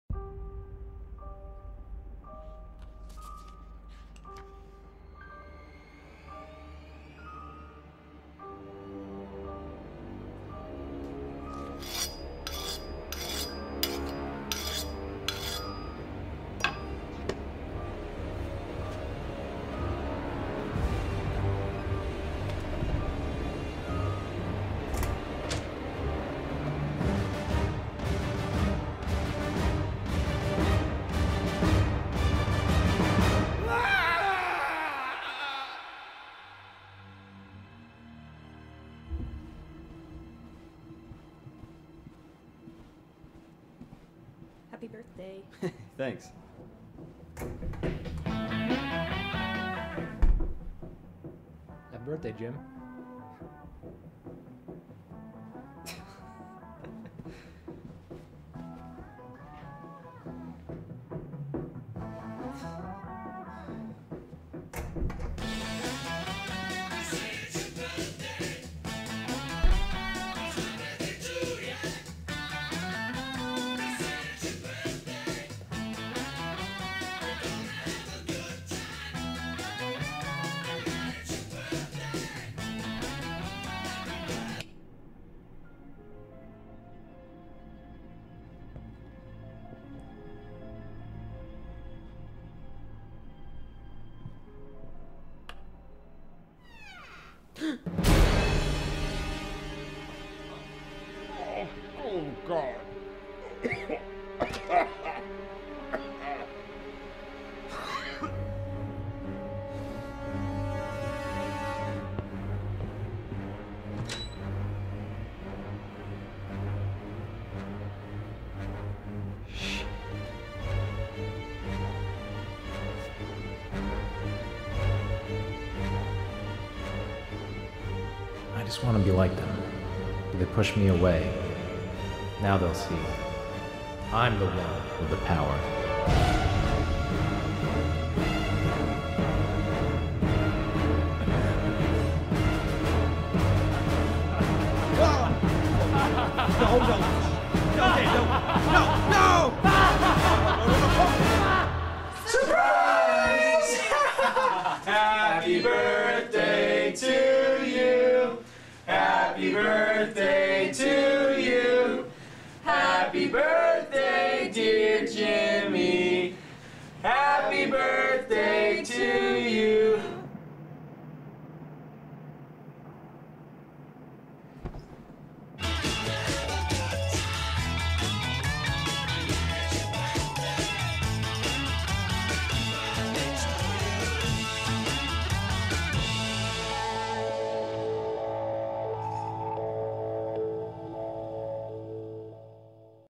This is Not a Joke
Jim returns to his apartment to find an unusual birthday surprise.